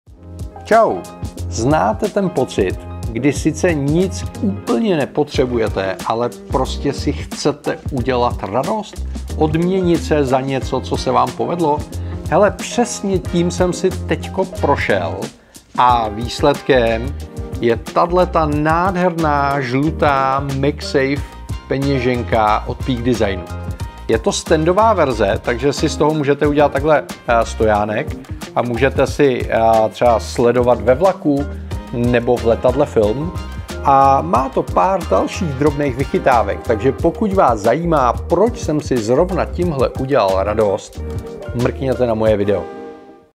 Nepotřebuješ ji, ale chceš.
Asi ji nepotřebujete, ale prostě chcete. PeakDesign peněženka je prostě super.

Ještě víc Apple informací?